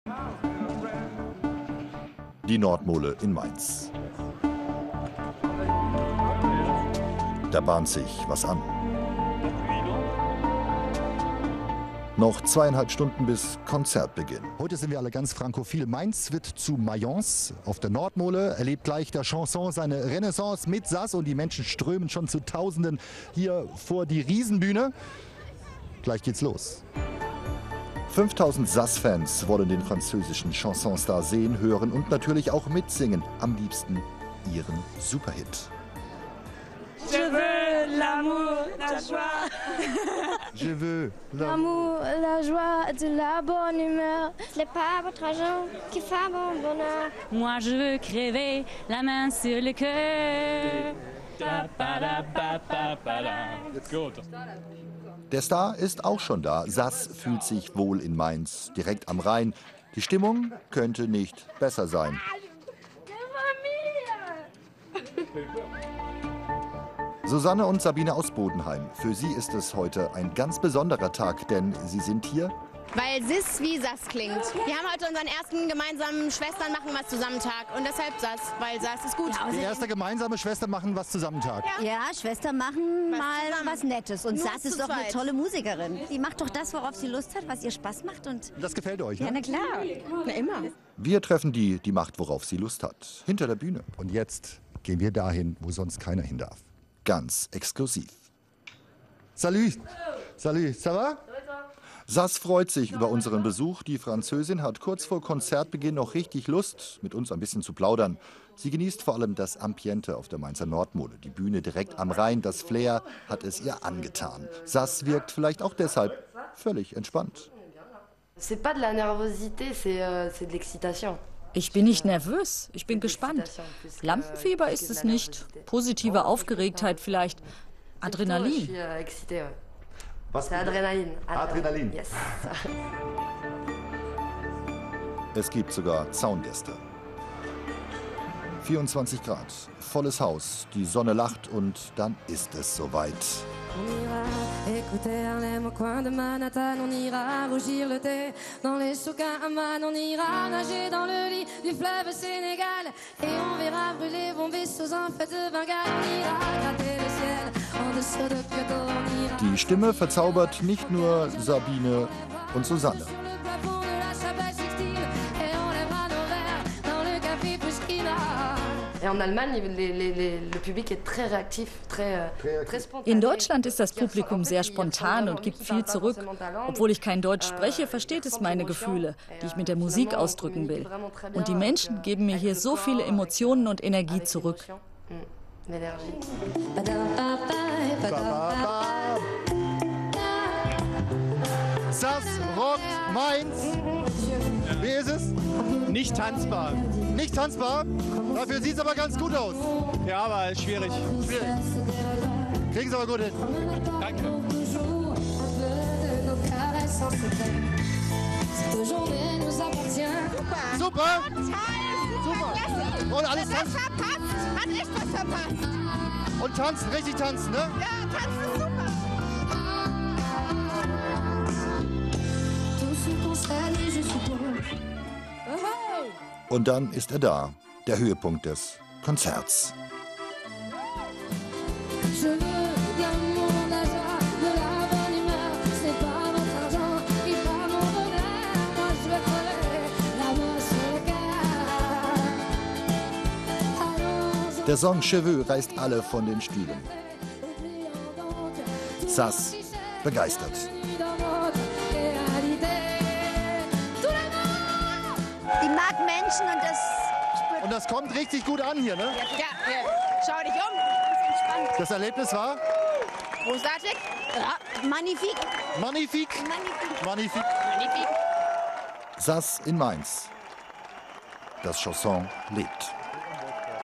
ZAZ begeistert
- Chansons mit Einflüssen von Jazz, Pop und Rock - das ist die Musik von ZAZ. Am Sonntag war sie in Mainz zu Gast. 5.000 Zuschauer waren begeistert und wir haben ZAZ exklusiv getroffen, mit ihr gesprochen und dann auf Ihre Musik getanzt.